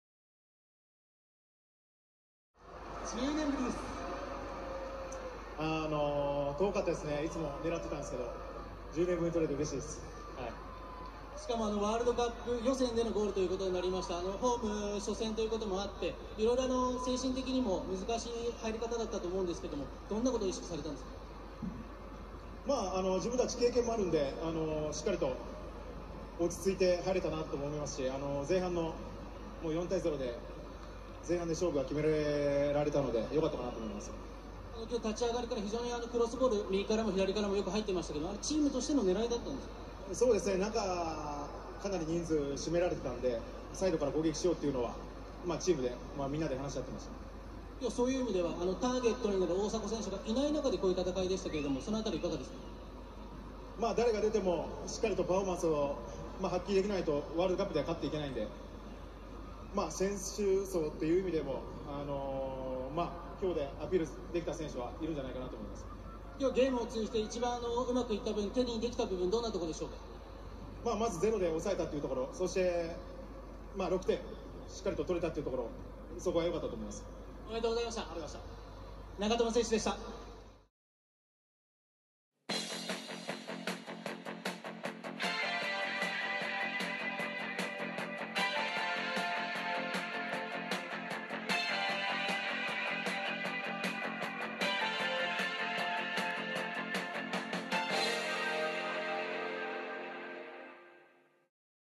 日本代表 長友佑都 10年ぶりの代表ゴール いつも狙っている インタビュー 2019/10/10 モンゴル代表
2019年10月10日、日本代表vsモンゴル代表
代表でのゴールは10年ぶりに決めた長友佑都のインタビュー。